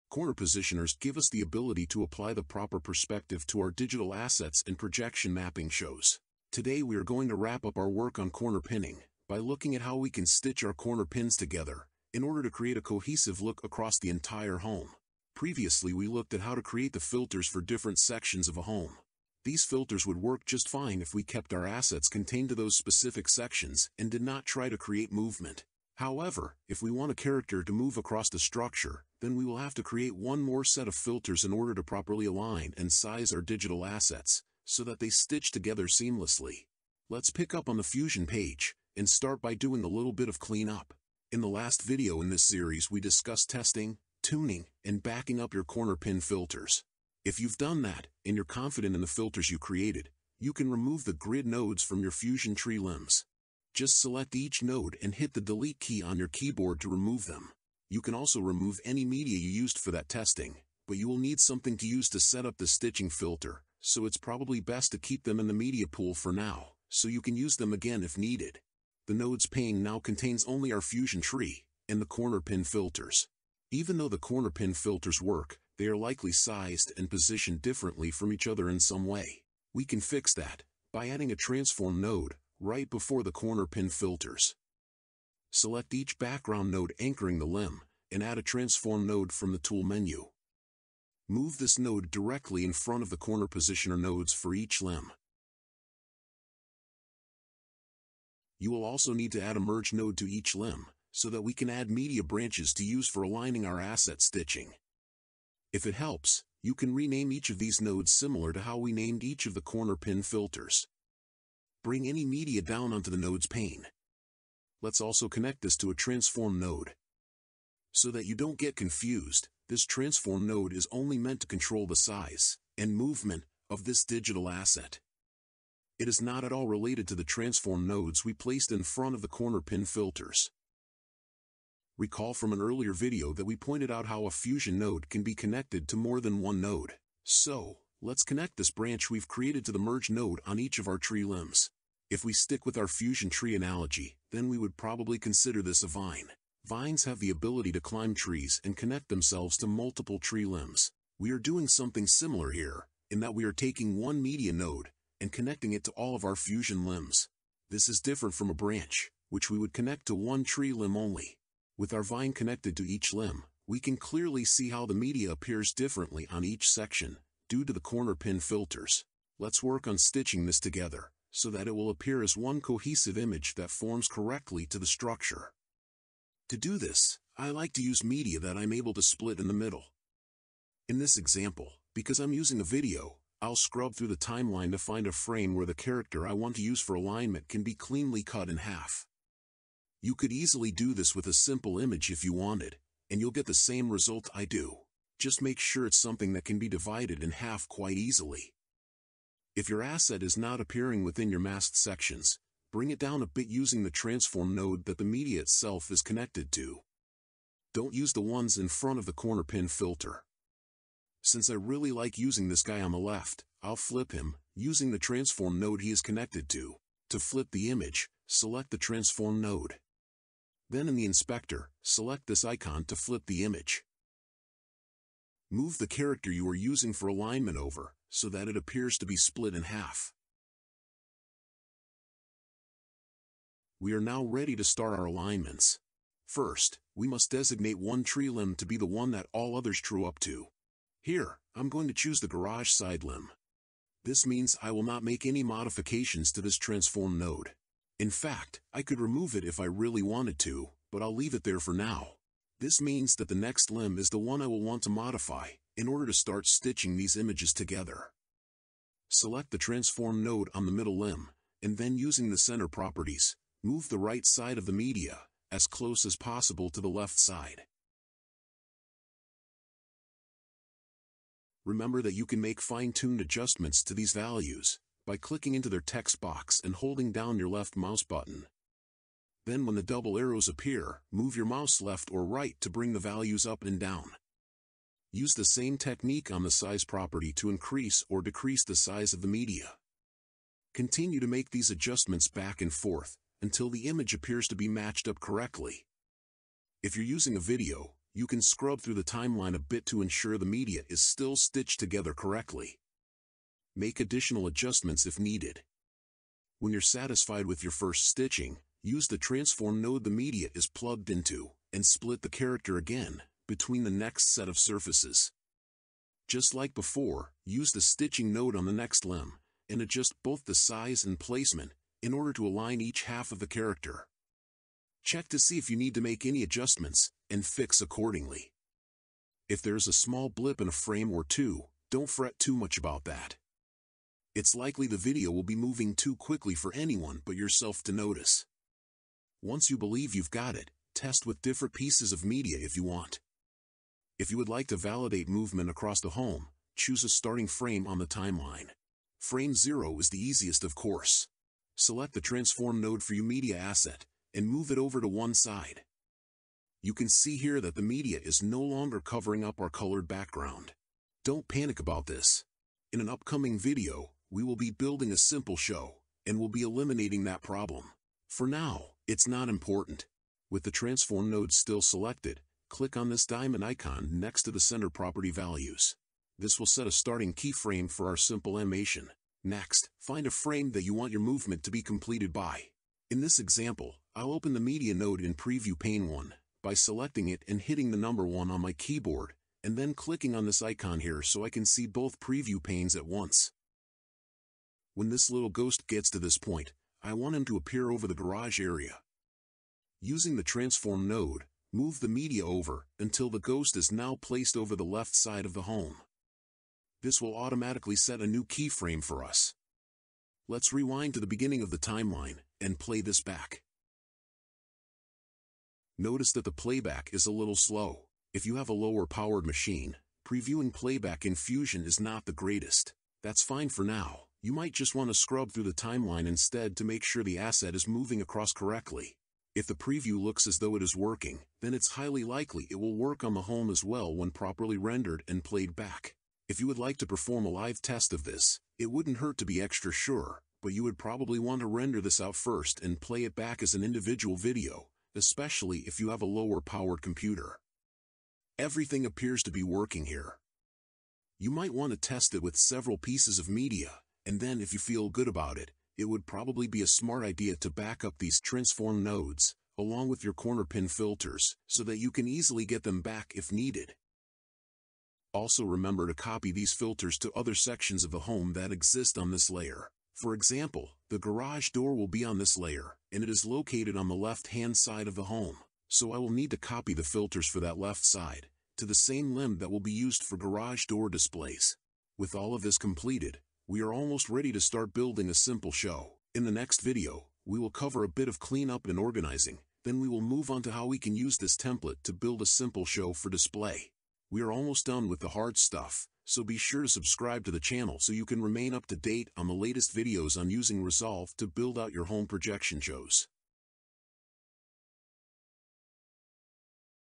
Resolve Projection Mapping Guide | Part 8: Stitching Corner Pins Together
🎬 Once you've created corner pin filters for each of the surfaces of your home, you'll need a mechanism that allows your digital assets to be displayed across the home as one cohesive image. In this tutorial we wrap up our work on corner pin filters (Finally!!!), and look at how we can create one more set of filters that will ensure assets can move across the home and appear to be the entire shape and size as they move through.

🎓 Some of the topics covered in this video include the following.
- Using Transform Nodes To Stitch Corner Pin Filters Together
- Connecting Assets To More Than One Fusion Tree Limb
- Simple Animation Of A Digital Asset (Key Framing)

🔧 Fusion Nodes Explored
- Background
- Merge
- Transform
- Corner Positioner

🔗 Relevant Links

🎓 About Resolved Home Projections: A passionate digital decorator specializing in home projection shows and Davinci Resolve. I'm just a fan of the hobby same as you.